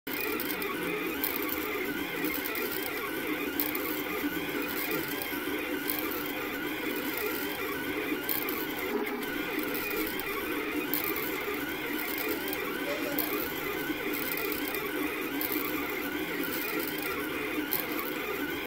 Rostock printing 50 mm/s with PLA
Airtripper's direct drive bowden extruder
Silver PLA 1.75mm from ProtoParadigm
MakerGear hotend with 0.5mm nozzle
Slic3r 0.8.3
--layer_height=0.3 mm
--perimeter_speed=50 mm/s
--travel_speed=400 mm/s
--scale=2 Minimug.stl
Marlin modified for realtime delta geometry on Arduino
Feeding regular G-code over USB using Pronterface